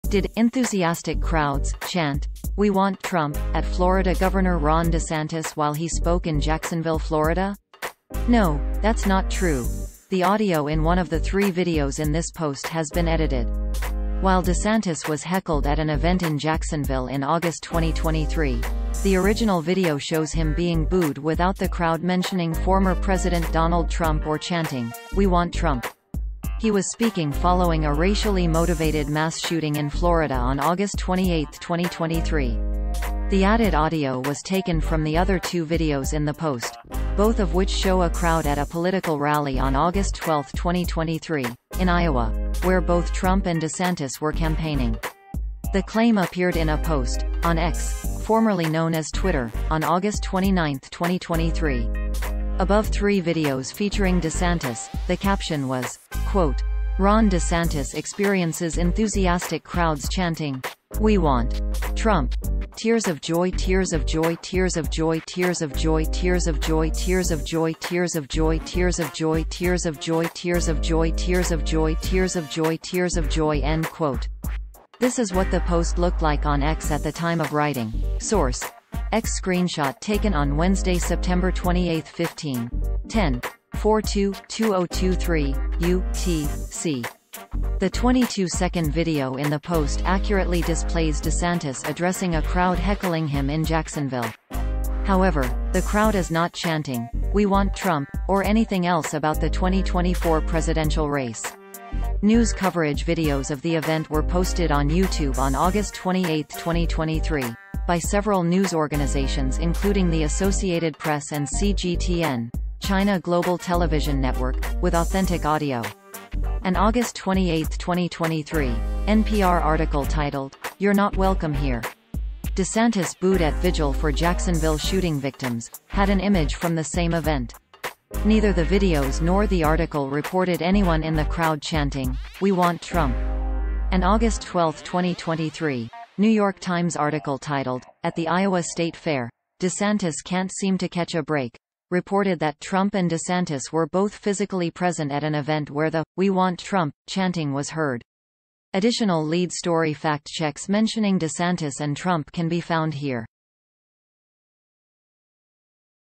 Fact Check: Video Does NOT Show Crowd Chanting 'We Want Trump' At DeSantis In Florida -- It Was...
FACT CHECK: VIDEO DOES NOT SHOW CROWD CHANTING 'WE WANT TRUMP' AT DESANTIS IN FLORIDA -- IT WAS DIGITALLY ALTERED

Did "enthusiastic crowds" chant "We want Trump" at Florida Gov. Ron DeSantis while he spoke in Jacksonville, Florida? No, that's not true: The audio in one of the three videos in this post has been edited. While DeSantis was heckled at an event in Jacksonville in August 2023, the original video shows him being booed without the crowd mentioning former President Donald Trump or chanting, "We want Trump." He was speaking following a racially motivated mass shooting in Florida on August 28, 2023.